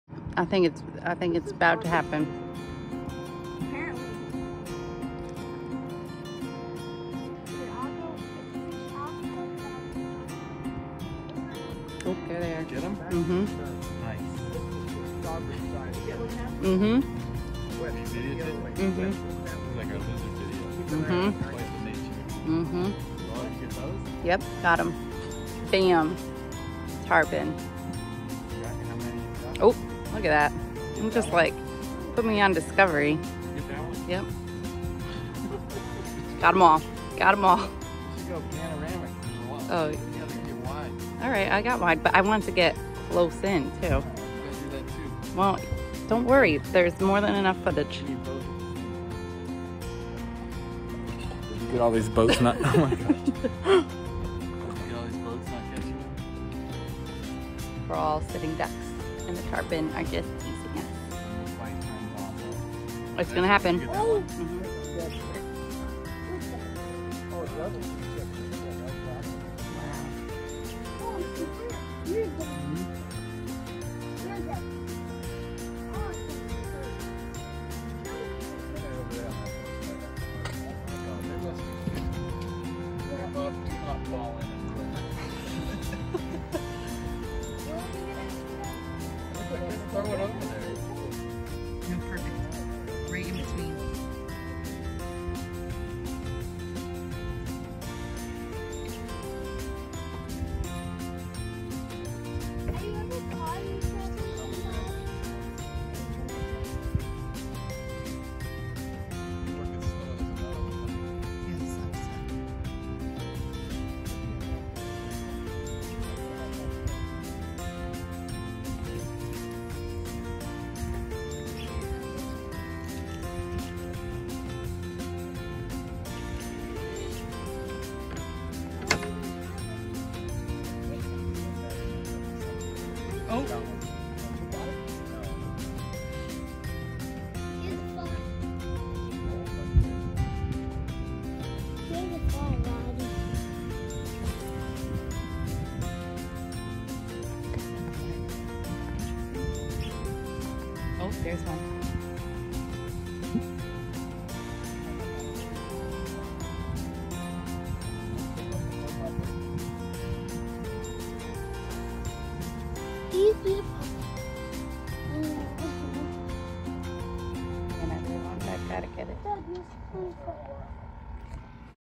Huge School of Tarpon Rolling in Florida
Huge school of tarpon rolling…but not biting. We saw one boat hook up but broke off pretty quick. Off Captiva Island, Florida.